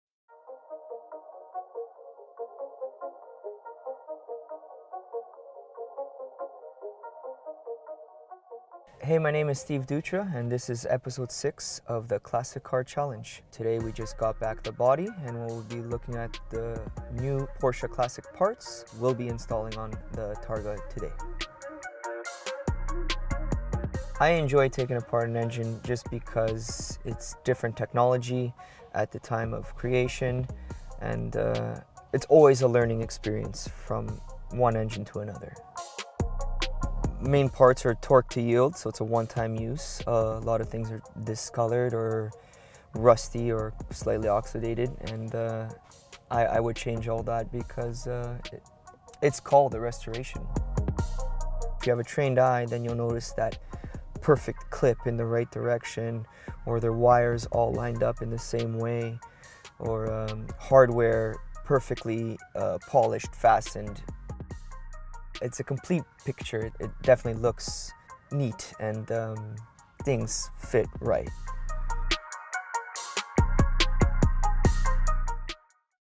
Chapter 6: The Details - Classic Restoration 2020: 1986 Porsche 911 Carrera Targa
In this episode, PCNT technician Steve Dutra discusses engine restoration.